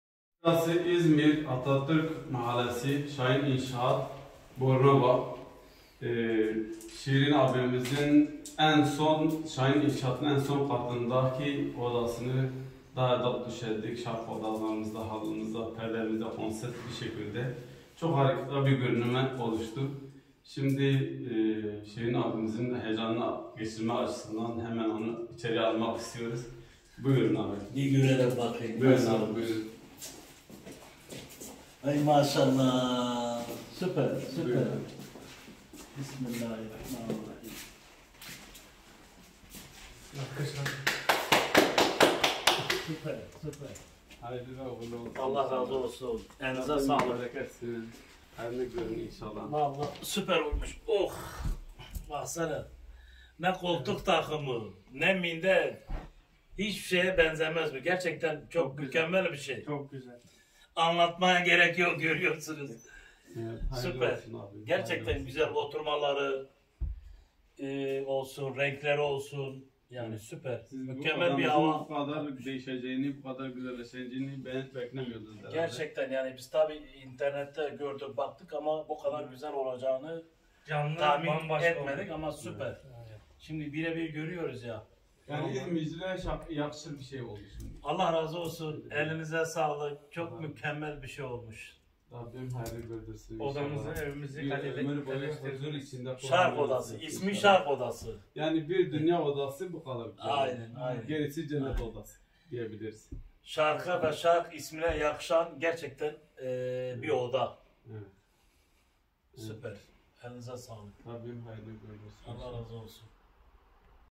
İZMİR BORNOVA
M.şirin çelebi Abimizin misafir odasını yaptıktan sonra oğulları Rıza ve Erhanada yaptık